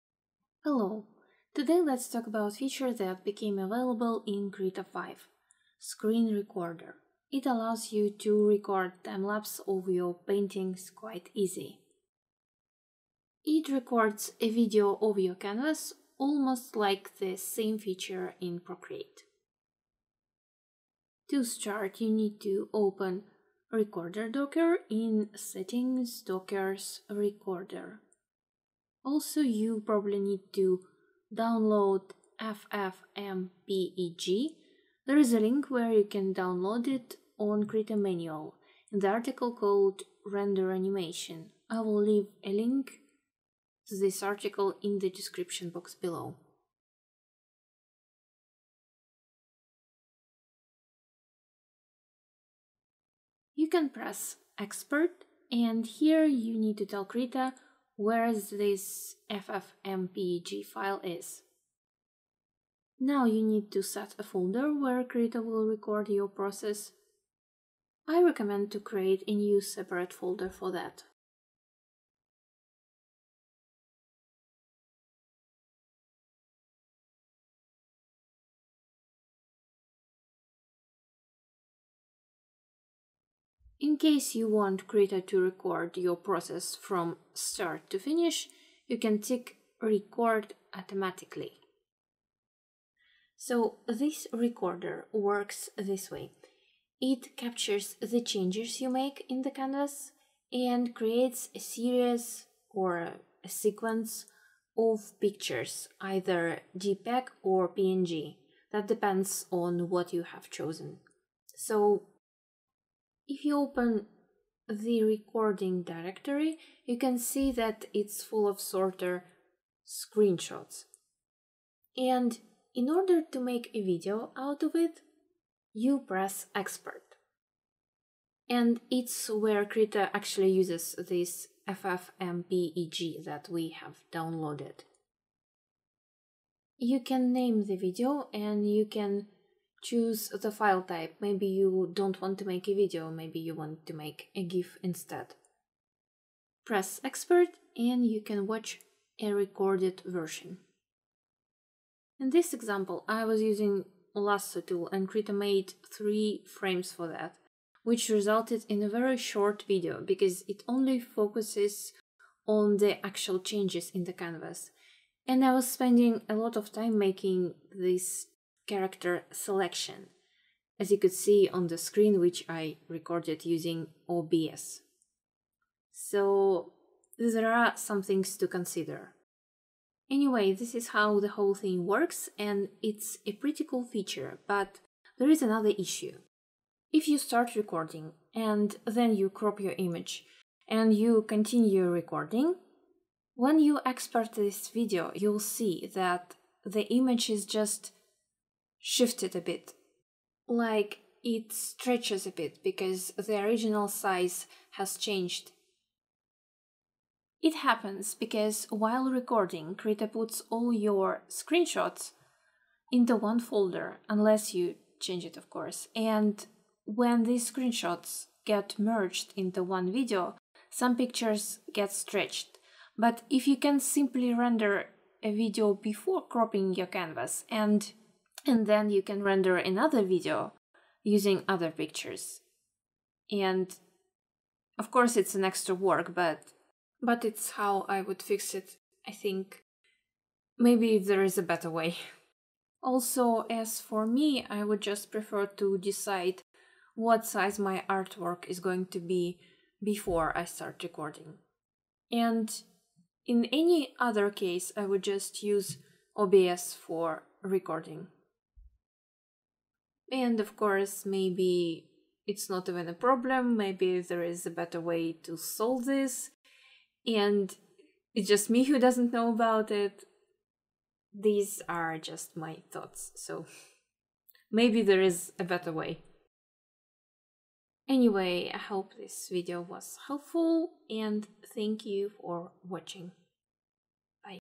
How to make a timelapse in Krita
In this video you can learn how to record timelapse recording in Krita.